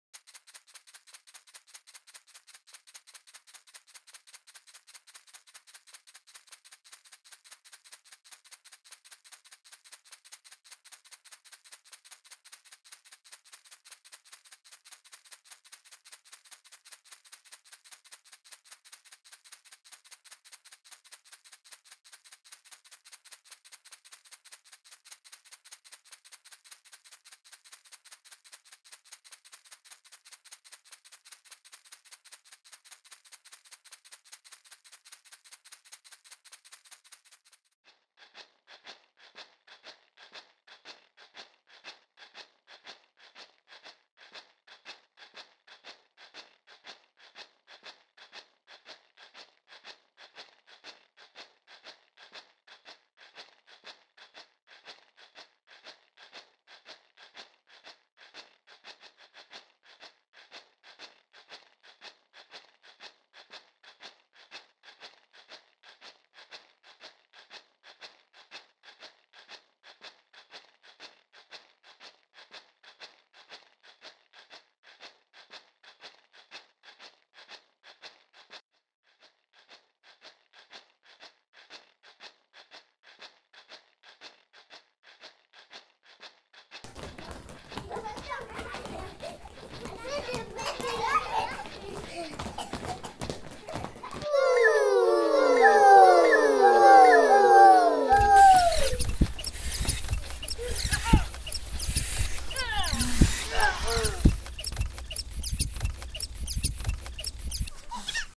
les gueules bleues, jaunes, vertes et rouges. JLEBAS Auberchicourt/// C.L.É.A Camille Gallard 2016
Les grands aux gestes et les touts petits font les sons ! Le cinéma c'est un travail d'équipe !